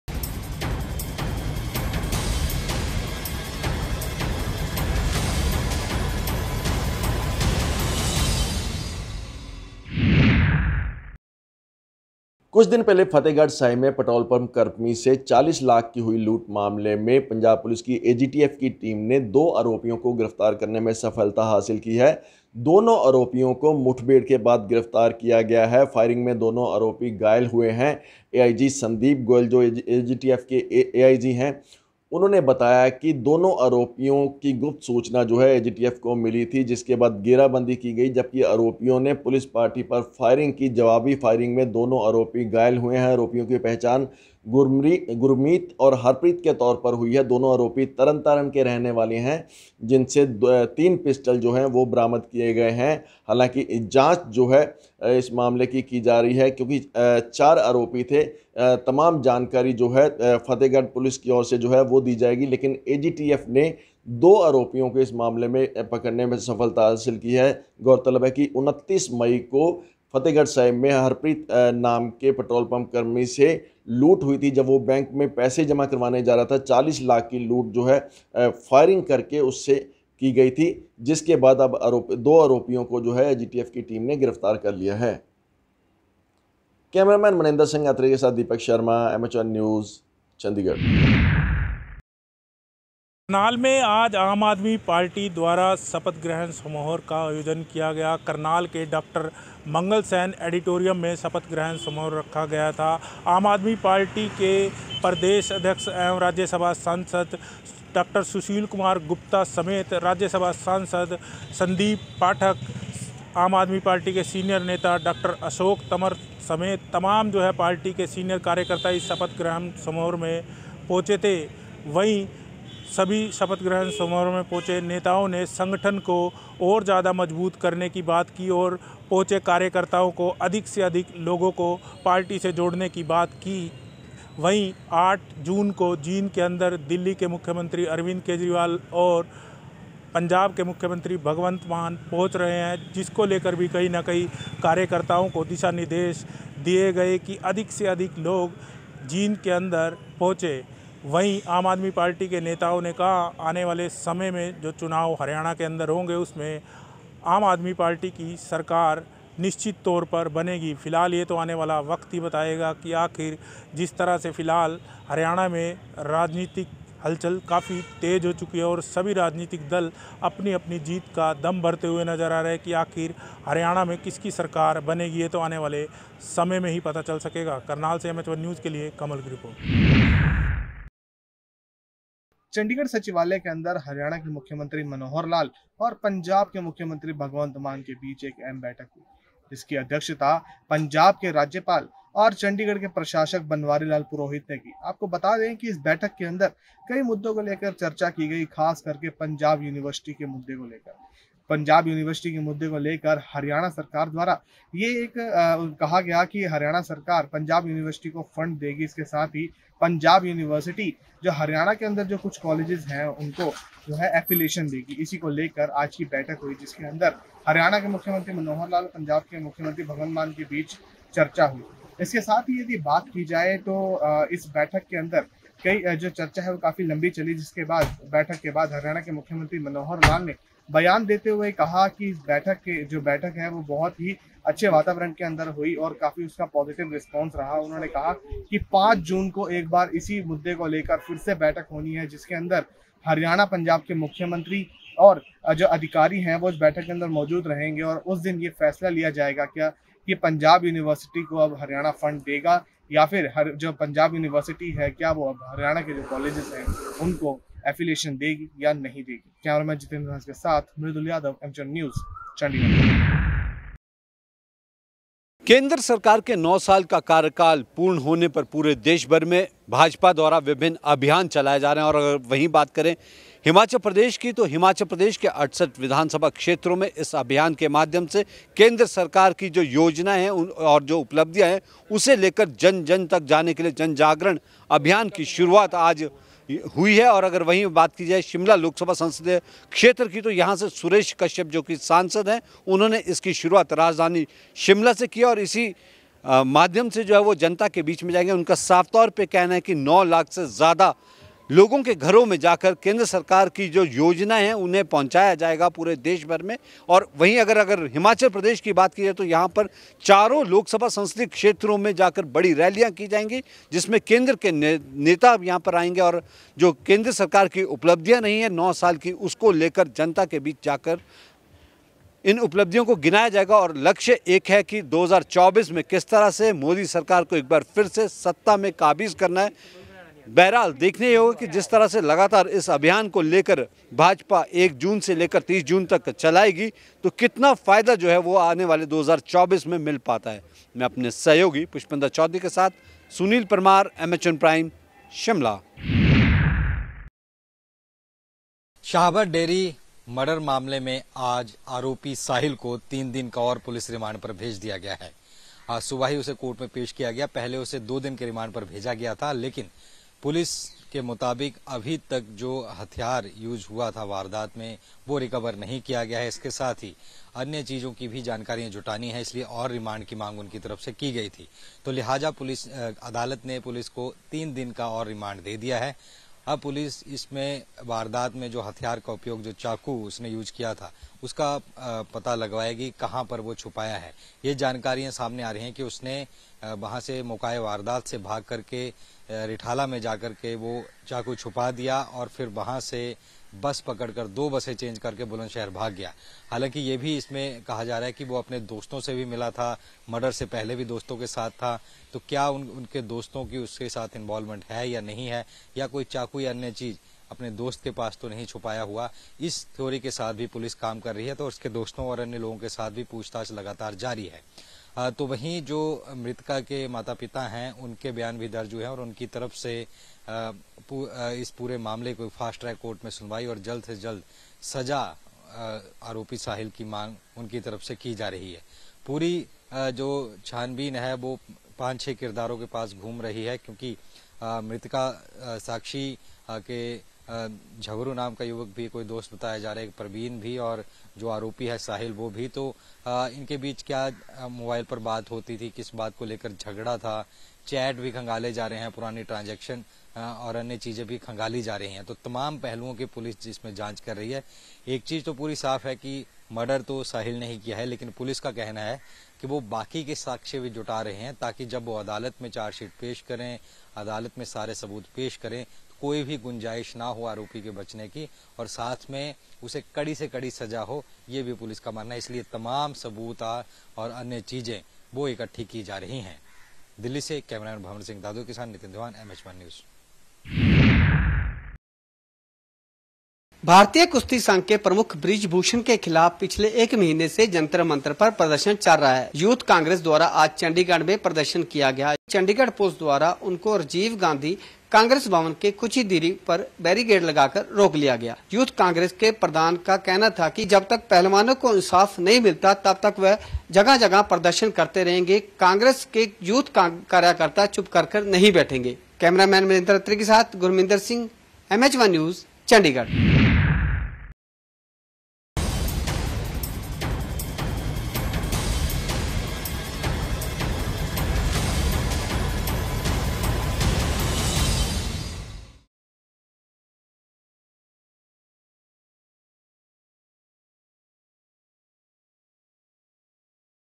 Ground Report: AAP की प्रदेश कार्यकारिणी का शपथ ग्रहण, राज्यसभा सांसद सुशील गुप्ता हुए शामिल
Ground Report: AAP की प्रदेश कार्यकारिणी का शपथ ग्रहण, राज्यसभा सांसद सुशील गुप्ता कार्यक्रम में हुए शामिल